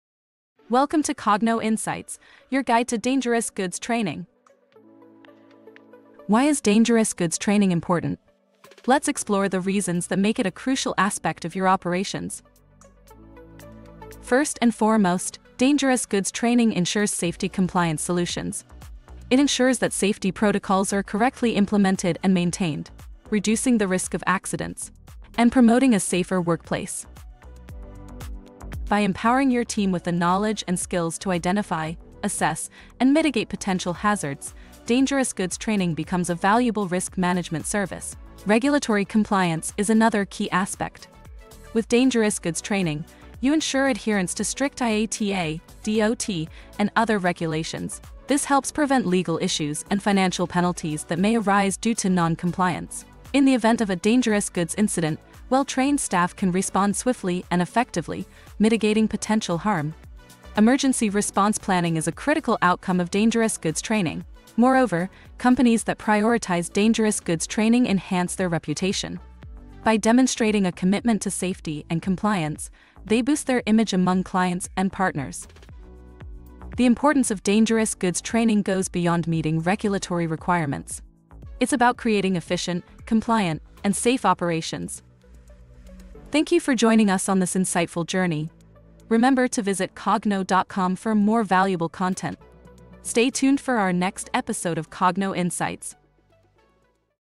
A Comprehensive Guide to Dangerous Goods Training Part 2 | Dangerous Goods Training 2023
In this video, we continue our comprehensive guide to dangerous goods training 2023

We will also go through some of the most recent laws and changes in the hazardous products transportation business.

This video is a must-watch for everyone who works with hazardous materials or is in charge of transporting them. It will give you the knowledge and skills you need to keep safe and in compliance with the rules.

Here are some of the benefits of watching this video:

You will learn about the different types of dangerous goods and how they are classified.
You will learn about the packaging requirements for dangerous goods.
You will learn how to mark and label dangerous goods containers.
You will learn about the documentation requirements for transporting dangerous goods.
You will stay up-to-date on the latest regulations and changes to the dangerous goods transportation industry.